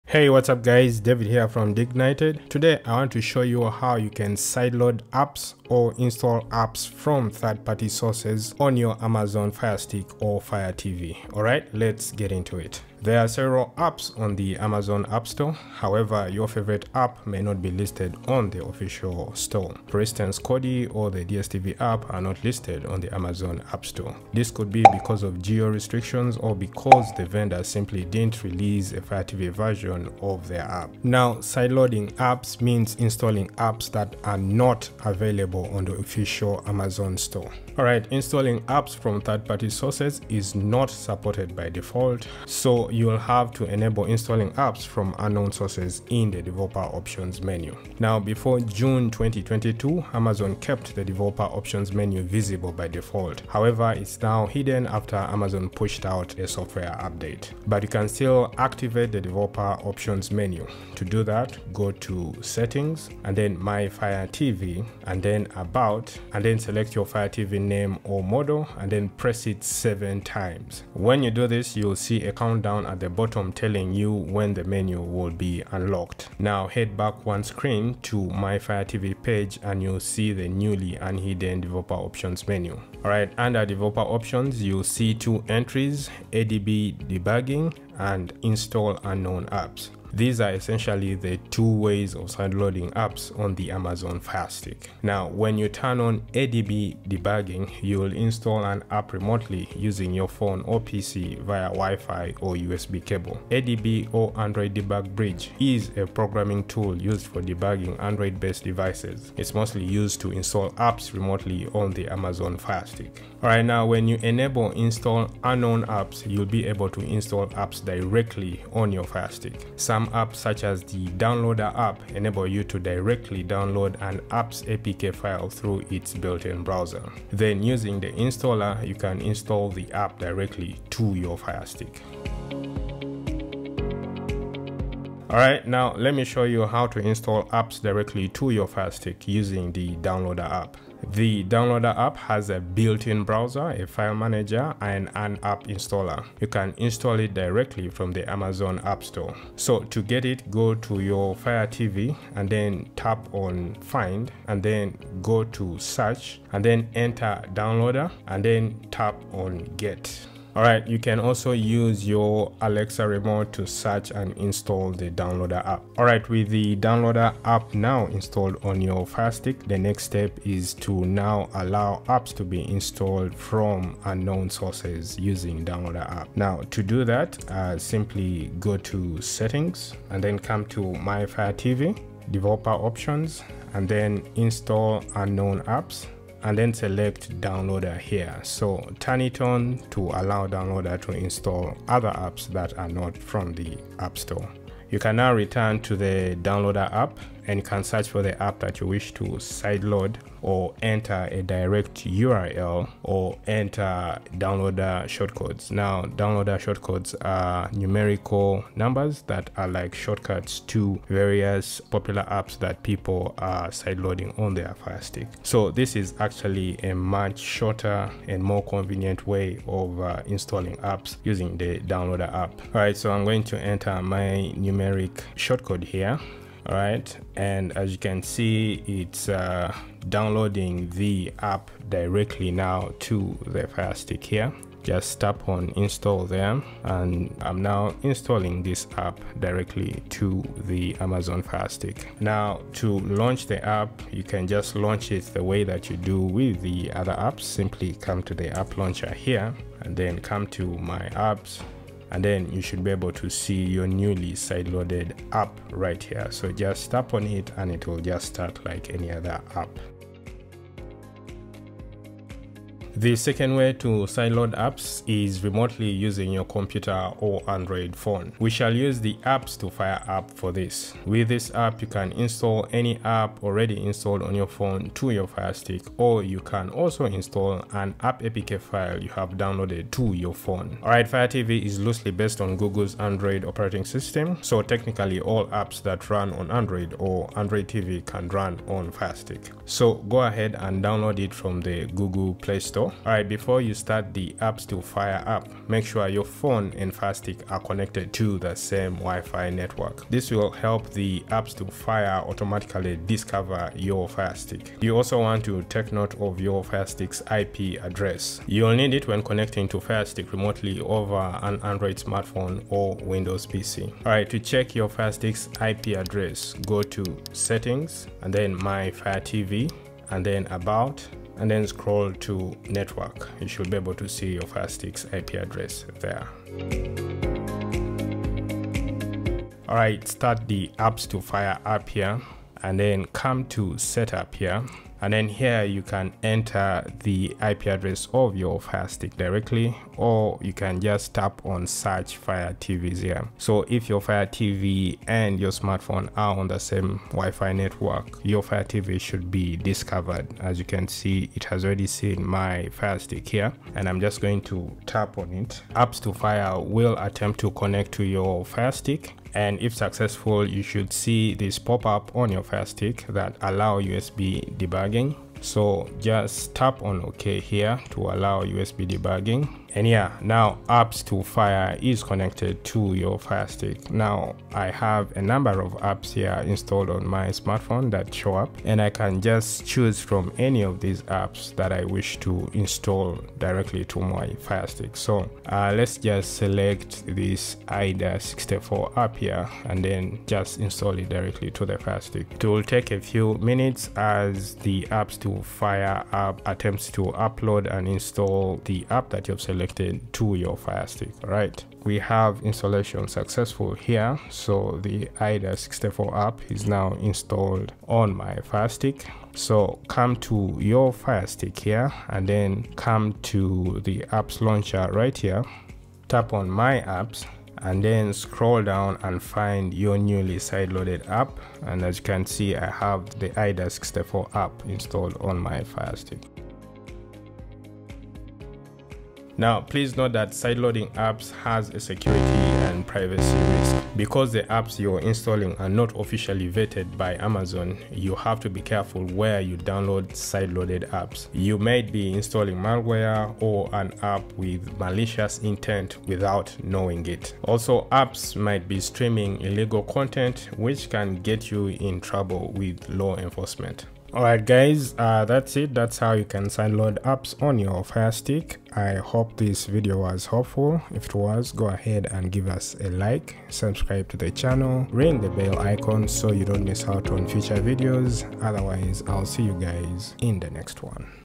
How to Sideload Apps on Fire TV Stick
Unlock the power of your Amazon Fire TV or Firestick by installing Apps such as Kodi outside of the official Amazon App store.